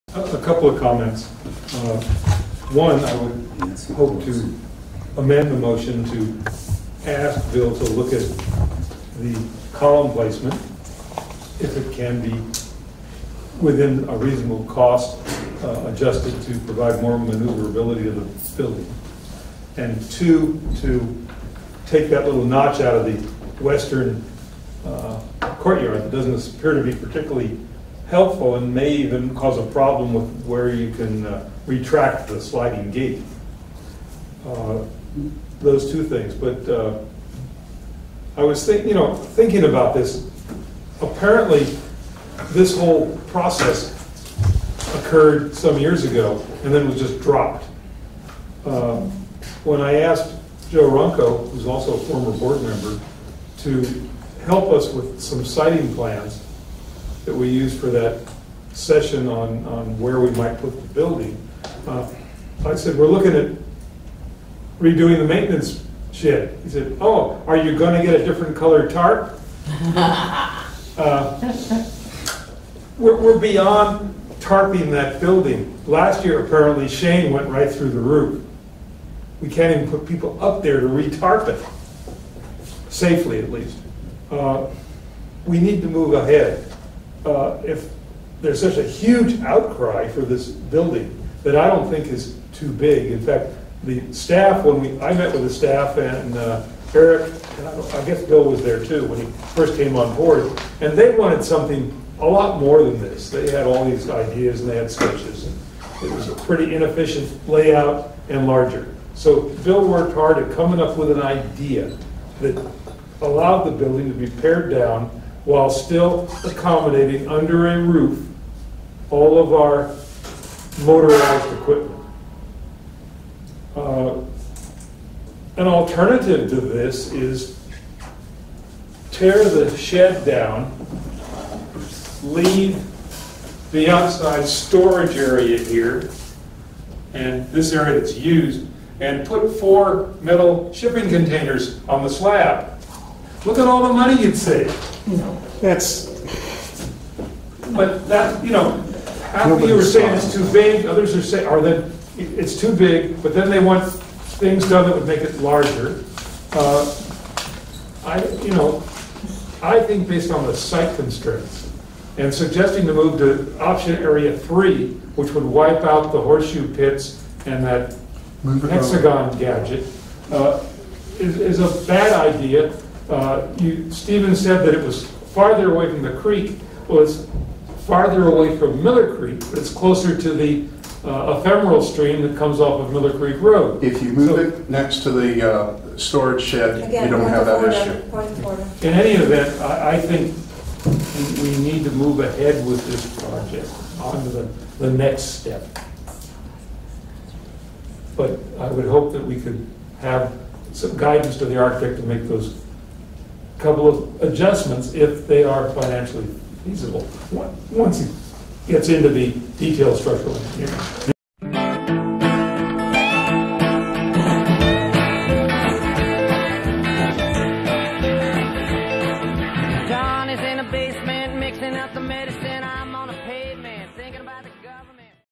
Irv Schwartz "Apparently " Forgets Business with Marinwood CSD
Irv Schwartz makes suggestions for improvements to the Hansell "White Elephant" Maintenance Compound which are helpful.  "Apparently" he forgets that his firm was the previous land surveyor for the previous Maintenance Shed initiative in 2010.  The effort was stopped when the CSD Business Manager discovered a $300k accounting mistake  which surfaced prior to the implementation of the previous design.  Irv Schwartz was also the developer in charge of the installation of the modular office in 2001.   He suggests absurd proposals as the alternative to the Hansell design that member of the public supports and uses as his reason to support the most over designed and expensive landscaping shed in Marin County history.